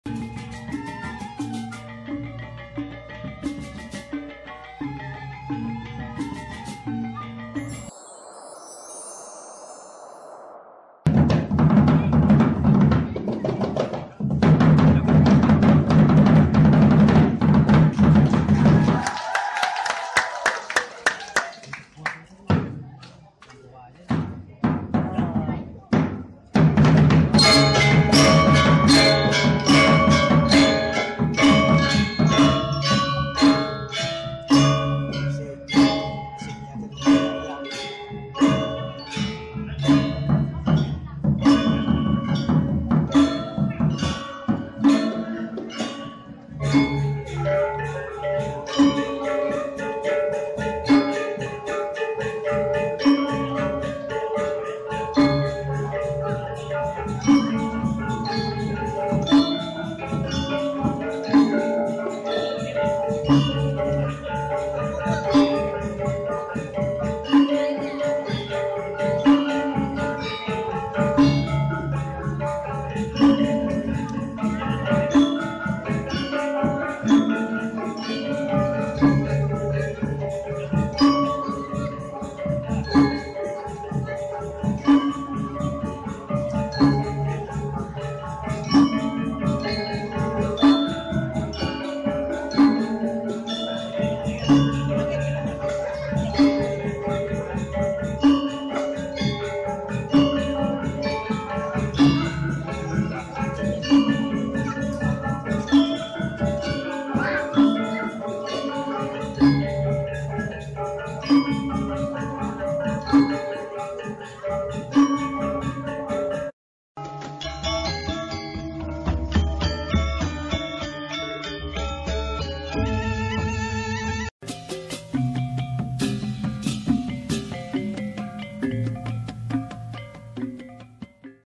Mantap aksi juara Festival Gong Kebyar Bali se Provinsi Banten. Luar biasa
Balinese Gamelan Festival. Mantap aksi juara Festival Gong Kebyar Bali se Provinsi Banten. Luar biasa. Let's watch and share before you travel to Bali for a real holiday and adventure in paradise.